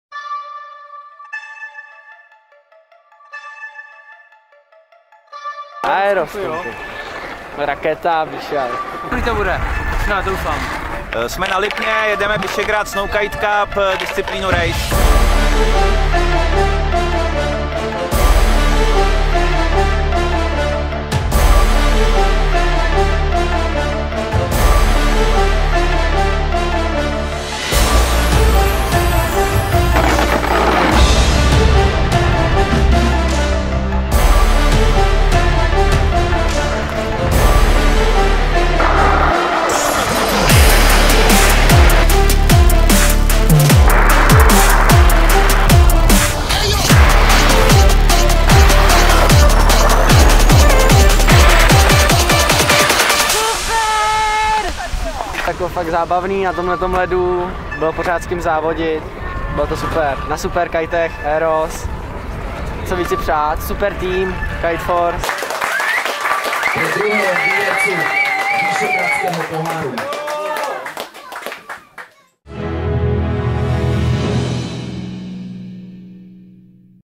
Visegrad snowkite Cup Lipno - tým KITEFORCE.cz
Krátký sestřih našeho týmu na závodech na Lipně, které jsme současně pořádali.
. . . . je nejúspěšnější závodnnní tým v Česku pro rok 2022.